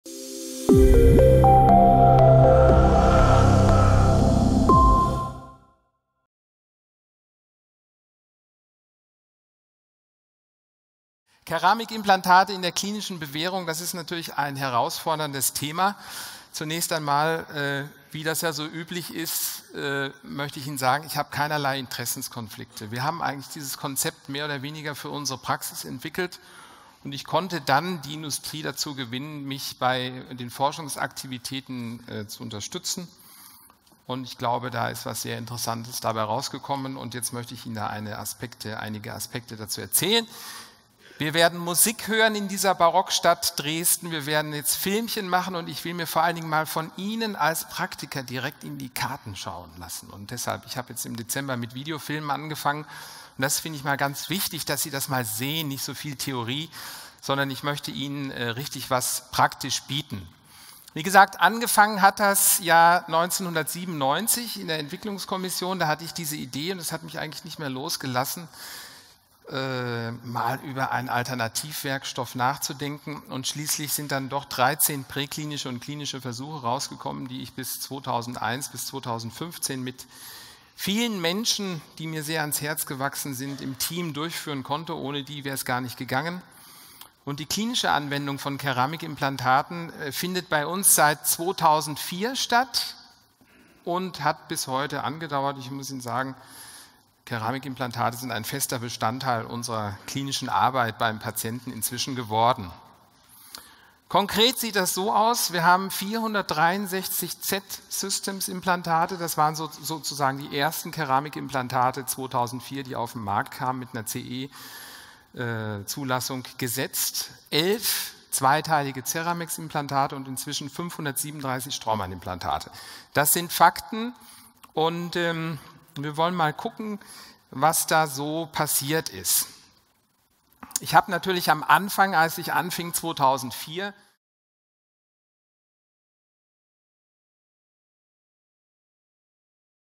Keramikimplantate in der klinischen Bewährung | Michael Gahlert
ITI Academy Congress Lecture Preview: Keramikimplantate in der klinischen Bewährung by]Michael Gahlert.